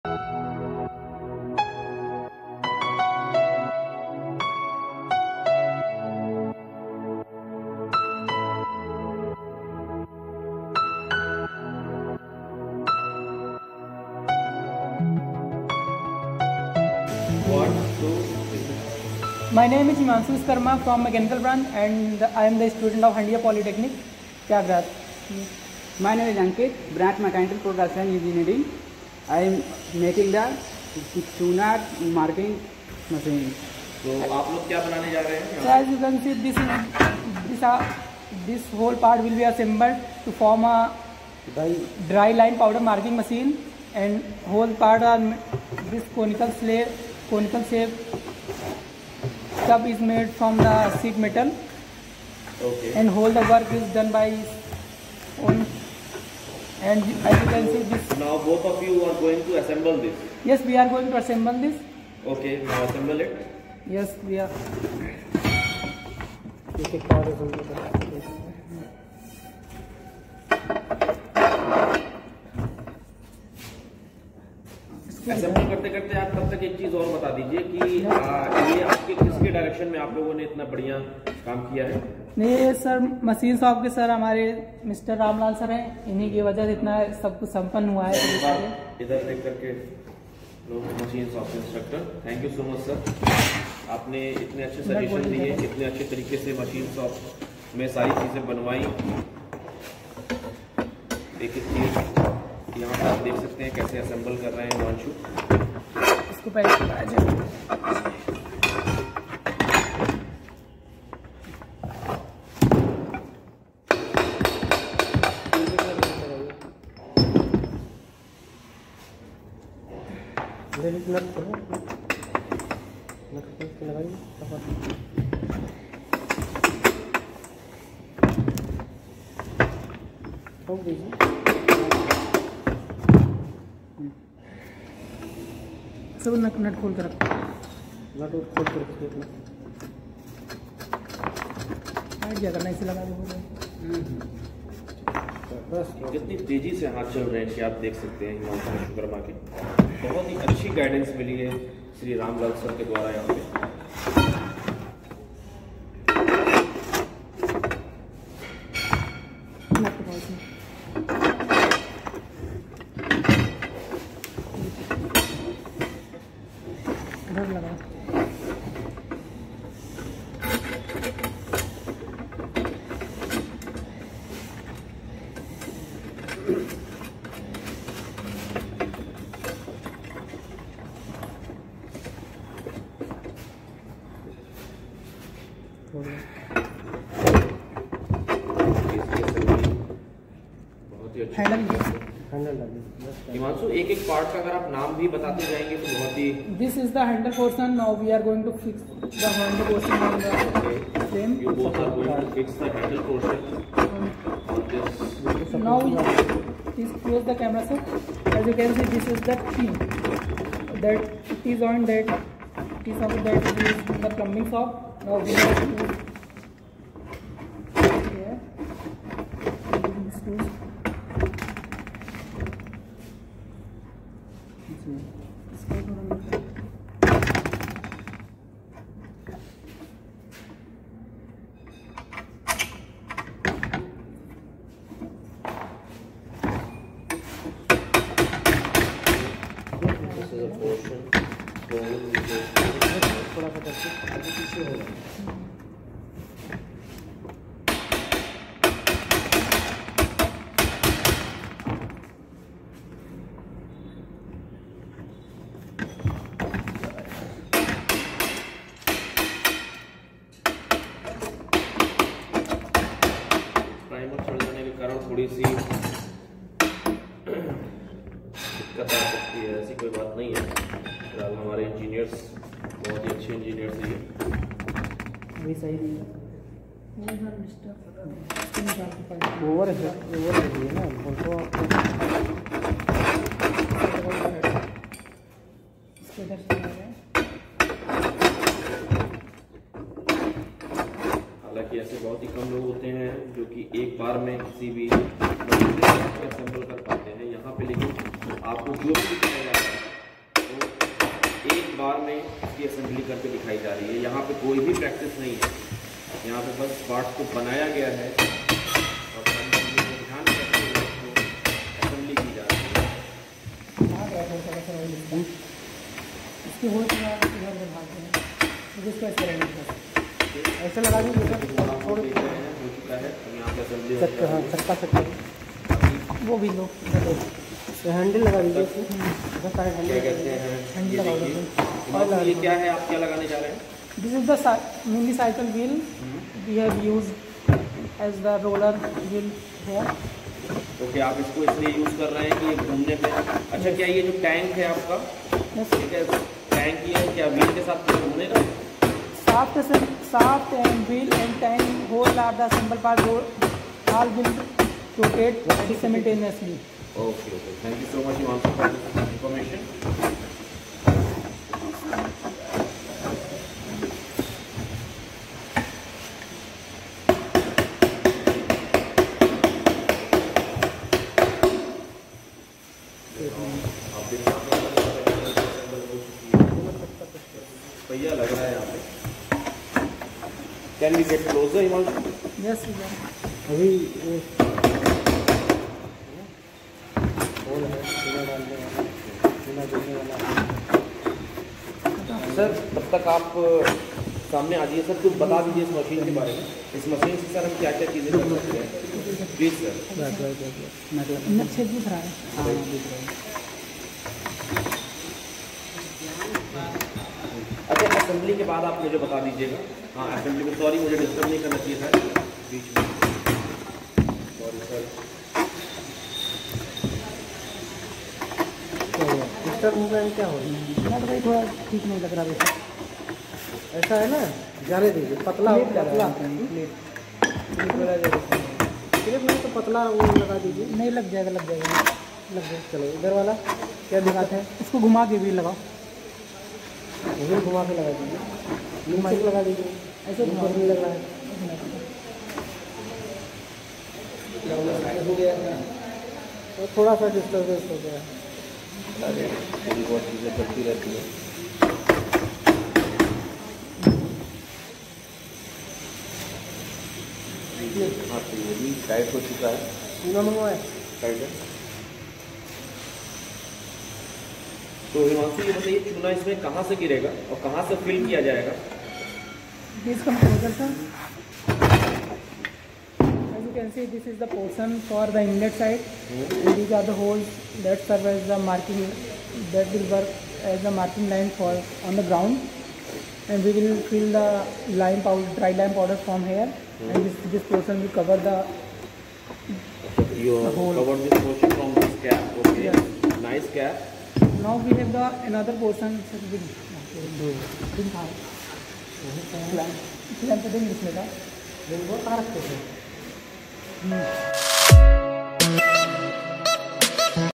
Dry lime powder marking machine
This is the assembly video of Dry Lime Powder marking machine which is made in Handia Polytechnic workshop.